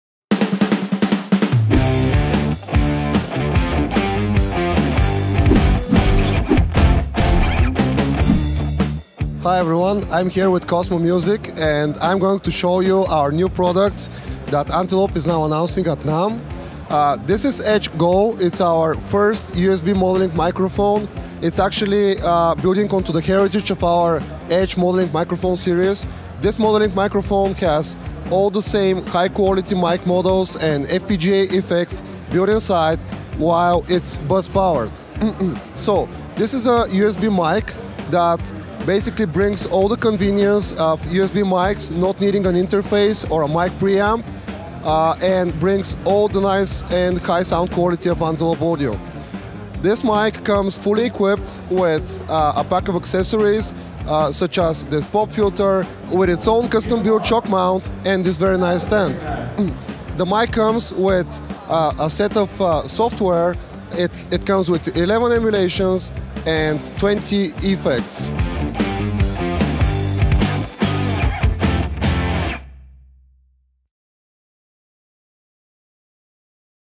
Antelope Audio Edge Go - Cosmo Music at NAMM 2019
Kris shows us the Antelope Audio Edge Go at NAMM 2019.